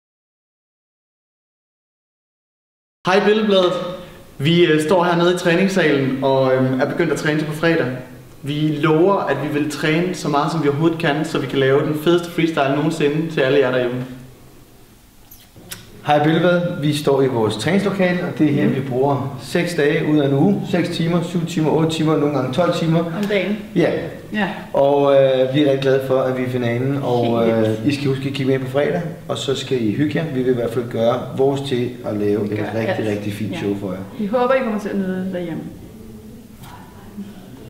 Se Vild med dans-finalisternes hilsen til BILLED-BLADETs læsere
Mie Skov og Mads Vad er i Vild med dans-finalen med Uffe Holm og Karina Frimodt. Se her, hvad de vil gøre for at løbe med sejren på fredag.